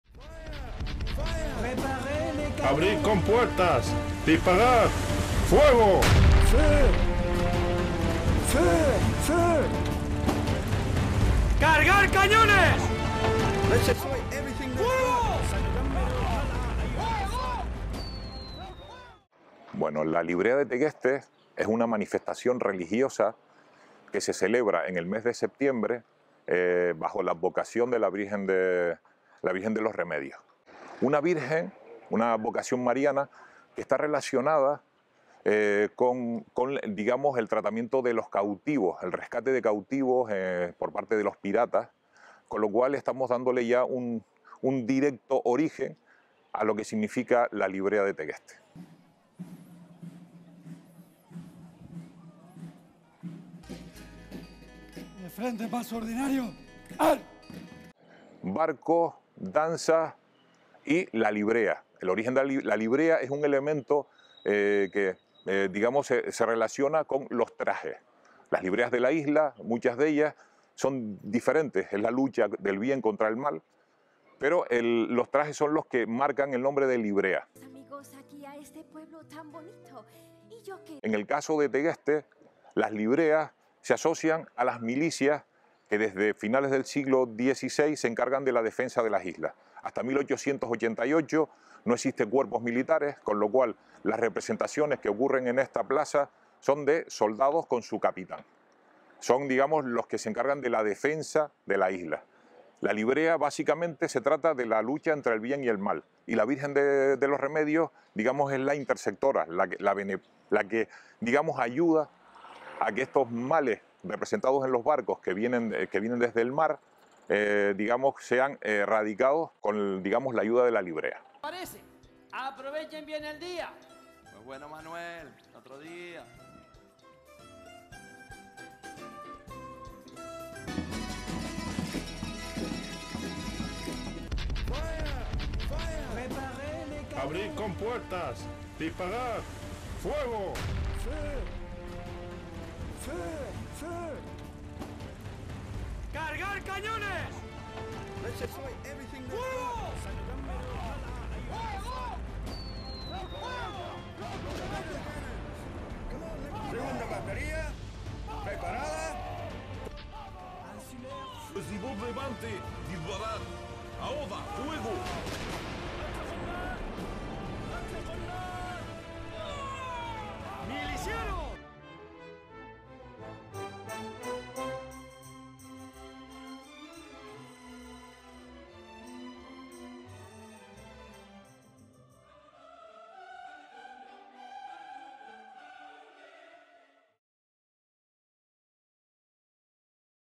La Librea de Tegueste. Tegueste, Tenerife, Islas Canarias.
La Librea de Tegueste. En la localidad de Tegueste, Tenerife, Islas Canarias. Fecha de celebración: 8 de septiembre, Día de la Virgen de los Remedios. Usos sociales, rituales y actos festivos

Esta fiesta ha sido declarada Bien de Interés Cultural y data del siglo XVII. Surgió de una serie de tradiciones con el objetivo de agradecer a los santos por proteger al pueblo de la peste o de los piratas. Los lugareños se ponen los trajes tradicionales de Tegueste para recrear escenas de la vida en el campo y las costumbres religiosas. Mientras tanto, los soldados marchan por las calles hacia la casa del capitán y se desata una batalla entre los barcos y el castillo, que ofrece una impresionante exhibición de luz y sonido. En la actualidad, la Librea se celebra cada cuatro años y se ha convertido en la fecha clave en el calendario festivo de la localidad de Tegueste.